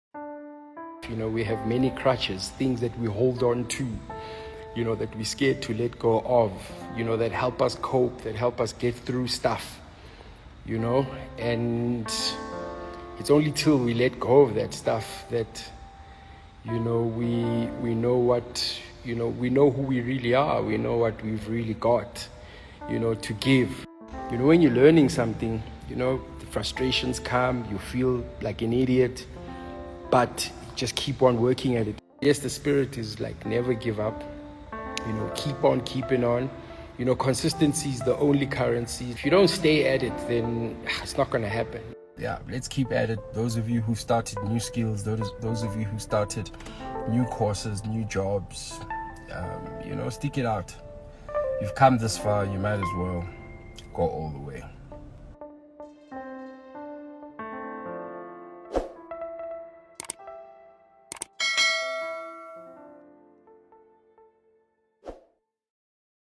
NEVER GIVE UP | Kabelo Mabalane | South African Motivation
Kabelo Mabalane known by his stage name as Kabelo or Bouga Luv is a South African kwaito musician, songwriter and actor. He was a member of the kwaito trio TKZee.